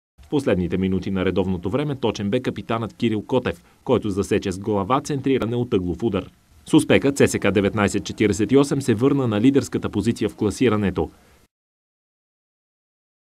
Кирил Котев - 11 гола с екипа на ЦСКА във всички турнири.
Кирил Котев има 11 гола с екипа на ЦСКА във всички турнири. 6 са за „А“ група, два за купата на България и два в Трета лига. Най-ценният обаче е този във финала за Суперкупата през 2008 година, когато ЦСКА победи 1:0. И най-интересното – всички голове са с глава!